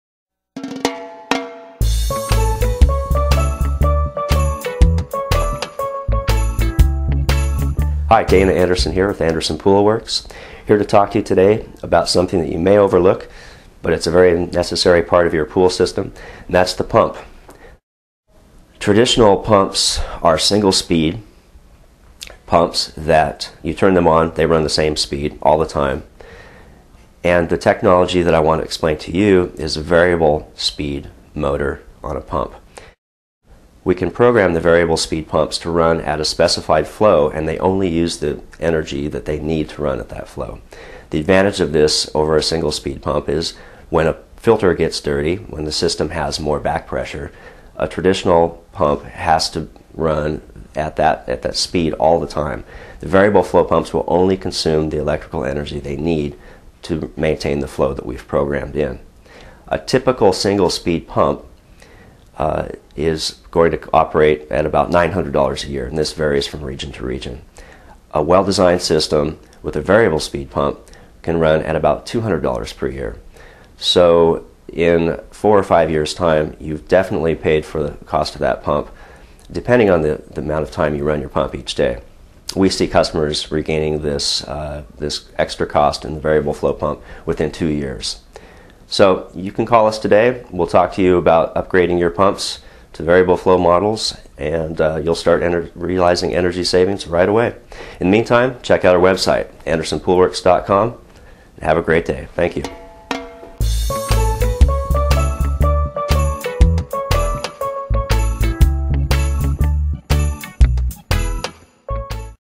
Variable Flow Pool Pumps
Dana Anderson, of Portland, Oregon based Anderson Poolworks, discusses the value that installing a variable flow pump will add to your pool.